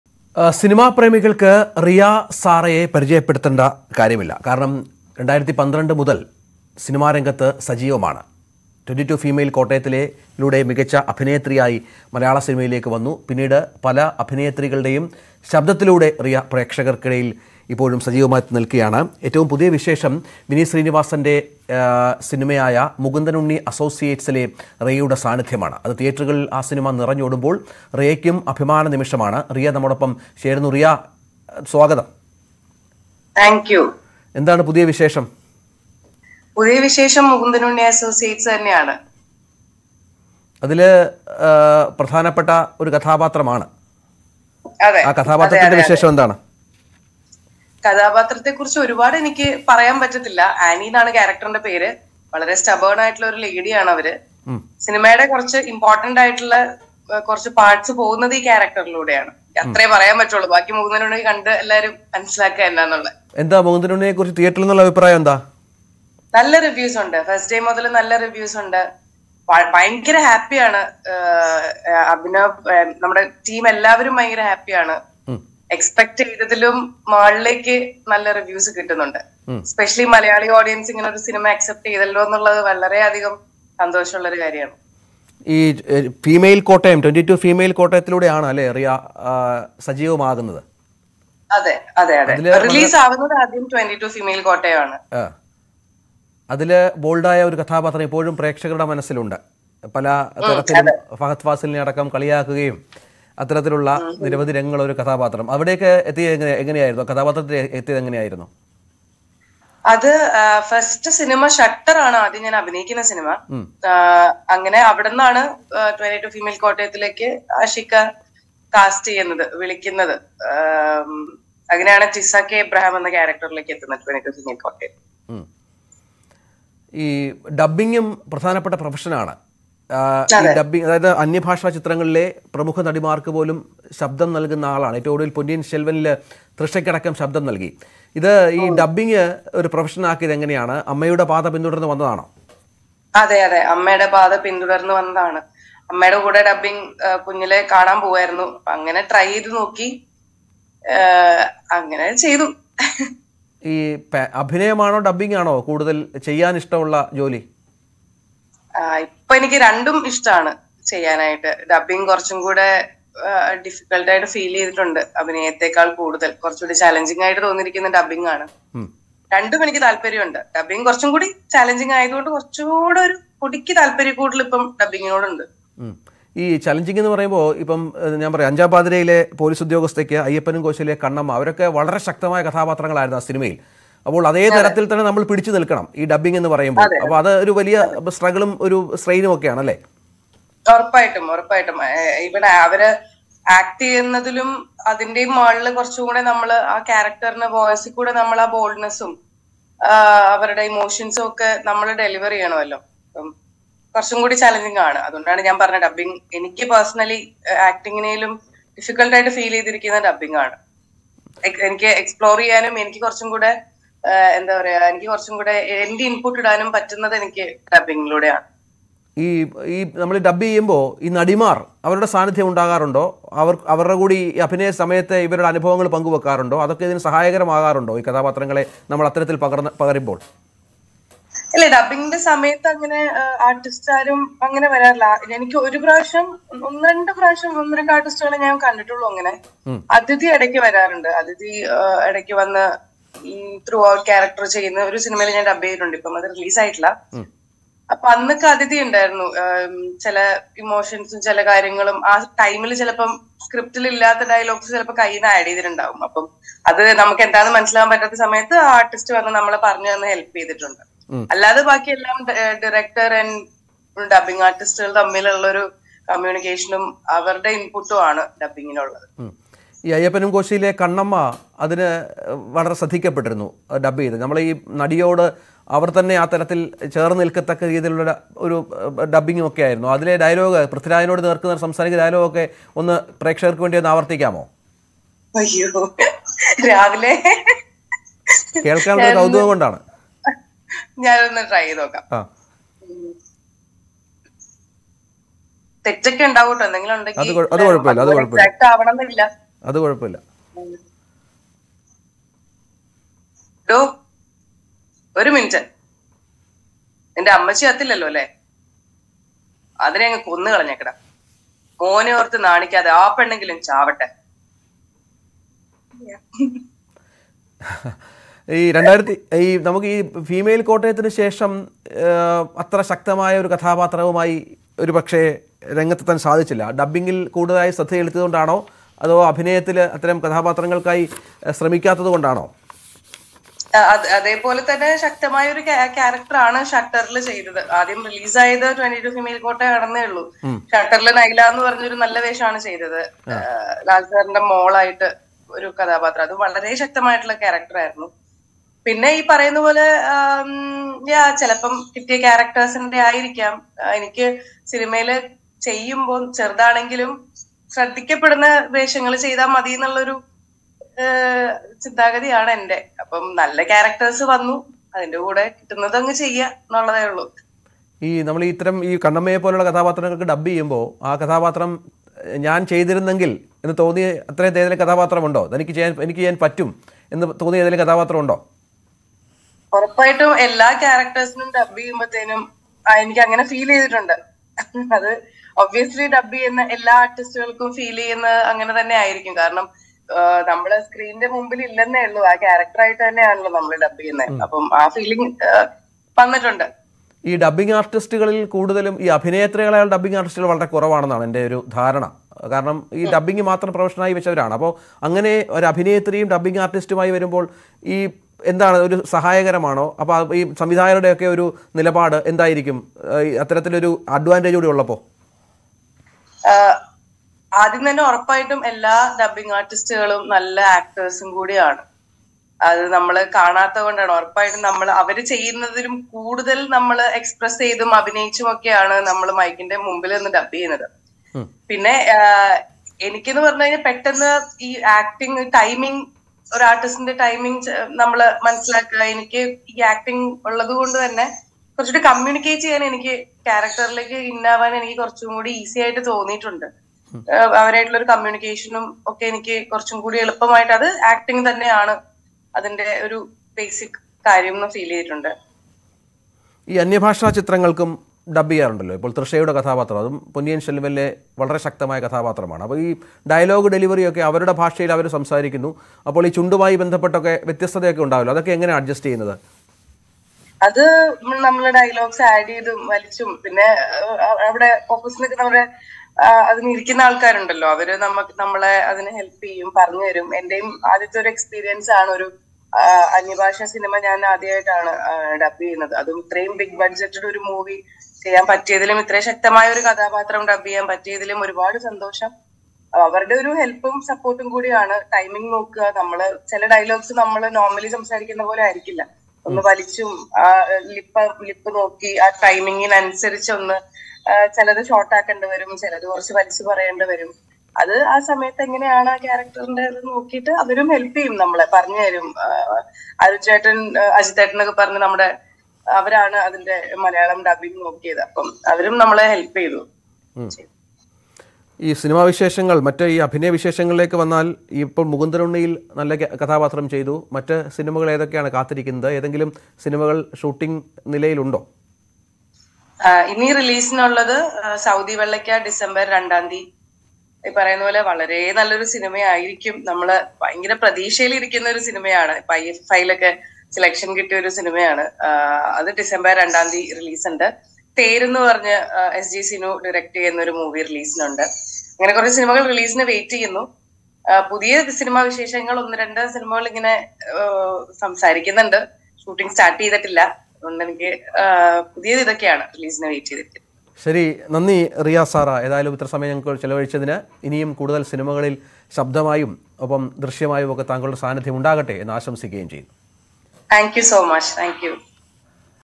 Riya Saira interview LIVE | Mukundan Unni Associates | Kerala | Malayalam Movie 2022 | News18 Kerala
Vineeth Sreenivasanഉം Tanvi Ramഉം കേന്ദ്ര കഥാപാത്രങ്ങളെ അവതരിപ്പിക്കുന്ന മലയാള ചലച്ചിത്രം Mukundan Unni Associatesന്റെ വിശേഷങ്ങൾ പങ്കുവെച്ച് നടി Riya Saira കാണാം ന്യൂസ്18 കേരളത്തിൽ.